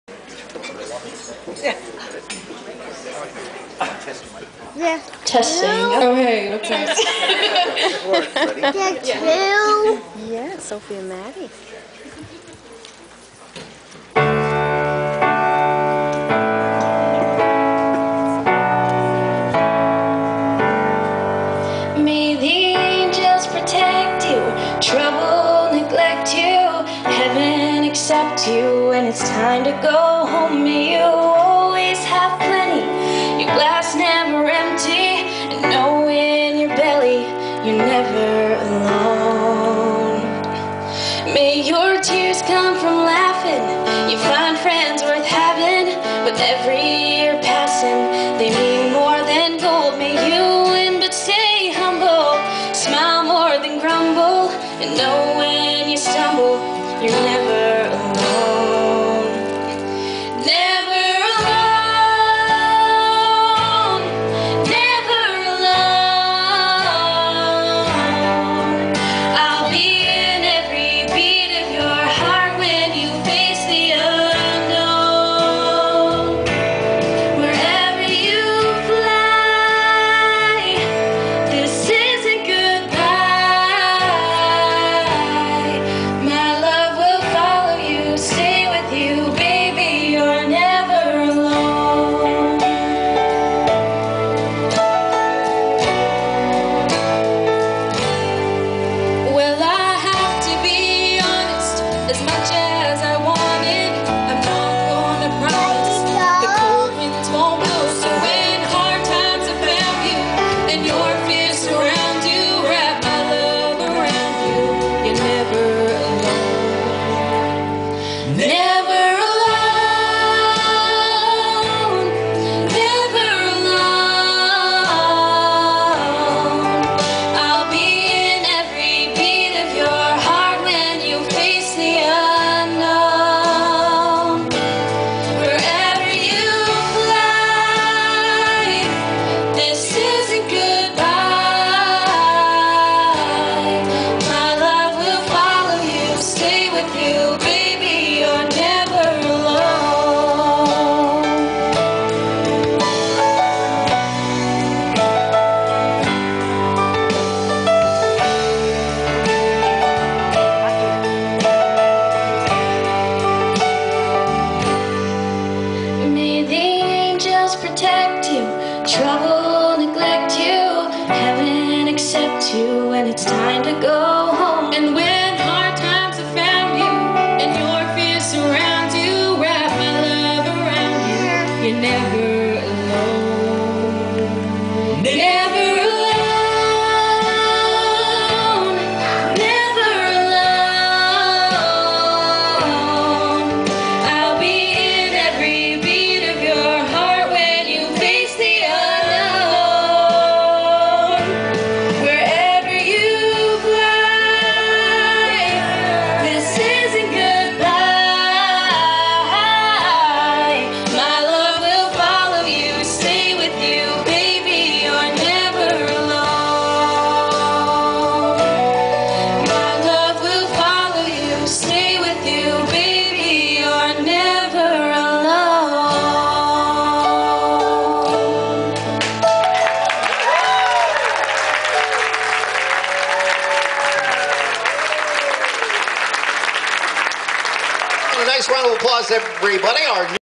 Alex's wedding-Maddy and Sophie singing the Bride and Groom dance.